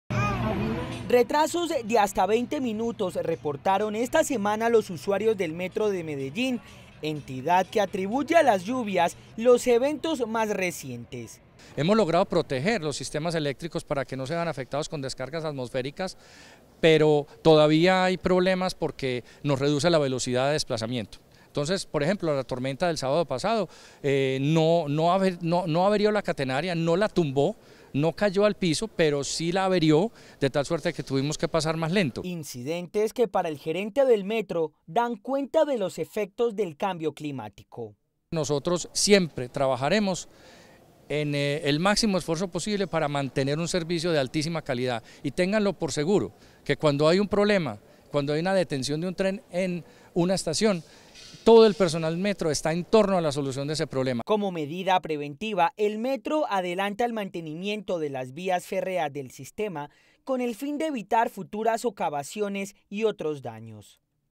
Lluvias continúan causando retrasos en la operación del Metro - Telemedellín
NoticiasTM NTM| Las lluvias volvieron a poner en riesgo una de las catenarias del Metro, lo cual generó retrasos en la operación.